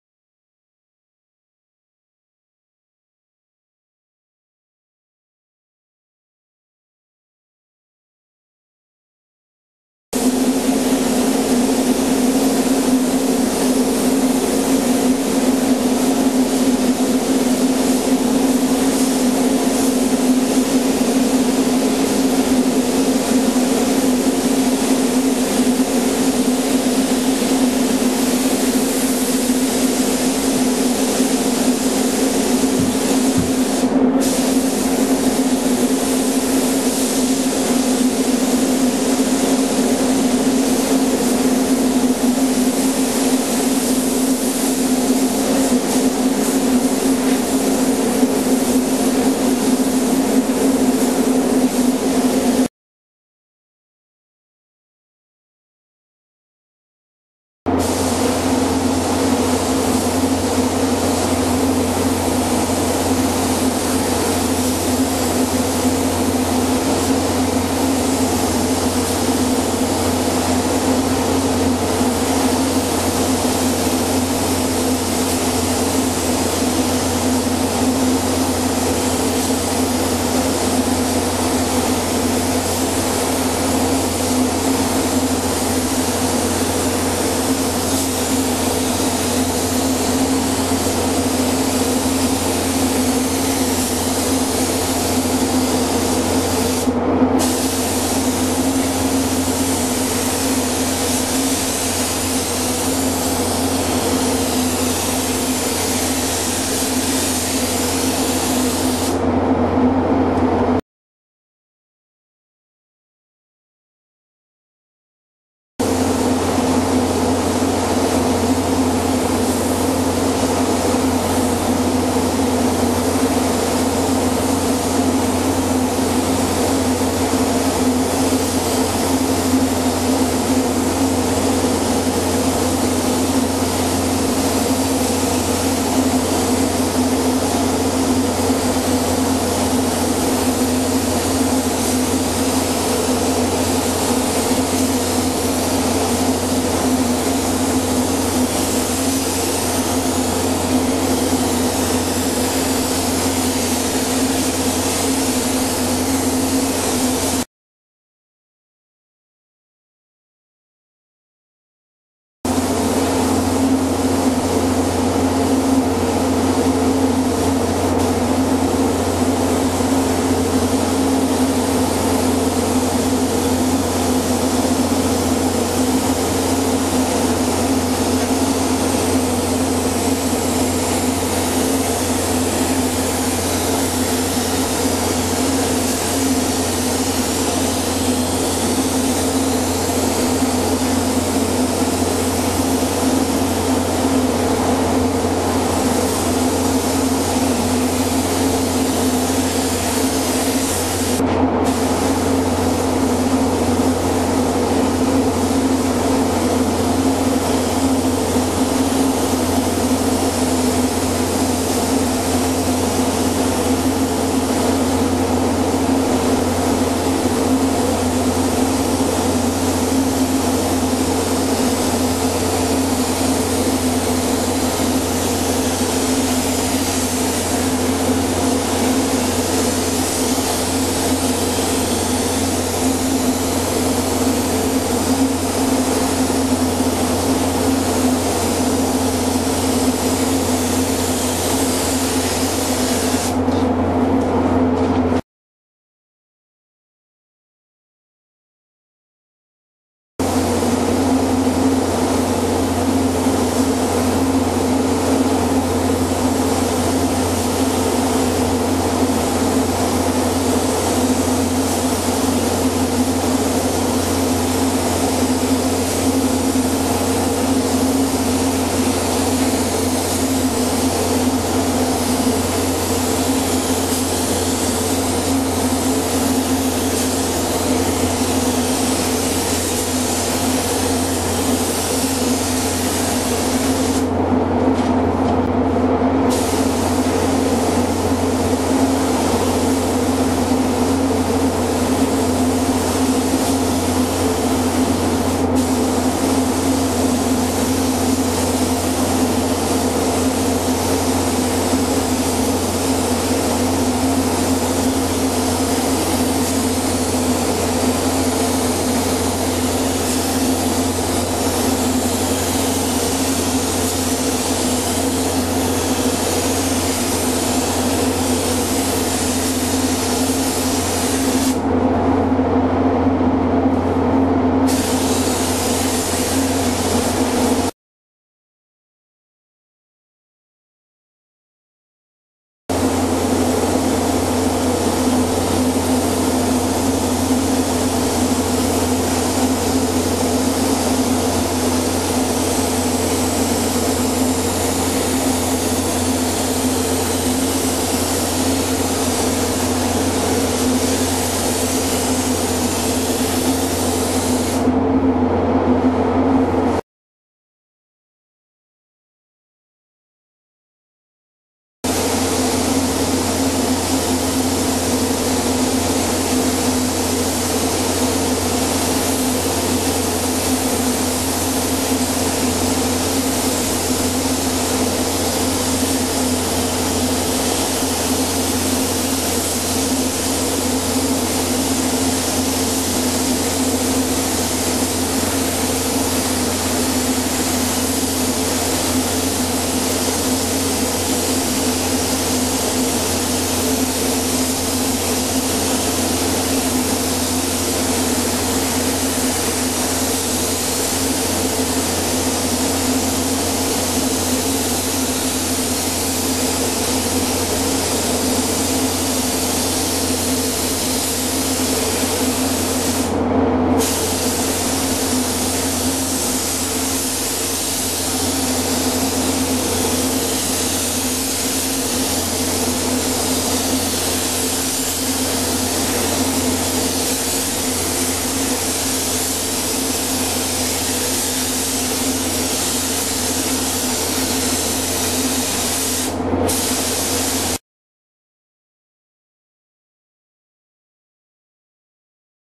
Custom Painting a Boat
This is a boat refinishing. Shown here as a three stage white with a candie stripe. It takes several paint steps to get the end result. Boat paint job.

Ive used a three stage pearl while on the top, a regular white on the bottom with a candy stripe around it.